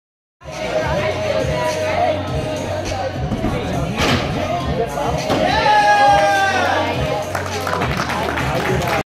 Warm-Up Vault (FTY) @ UMN Mock Meet 2015
My original goal back in September was to compete a Yurchenko Full by my first meet in November at Minnesota. My next goal will be to compete a Yurchenko Double Full by my next meet in February at Oklahoma :)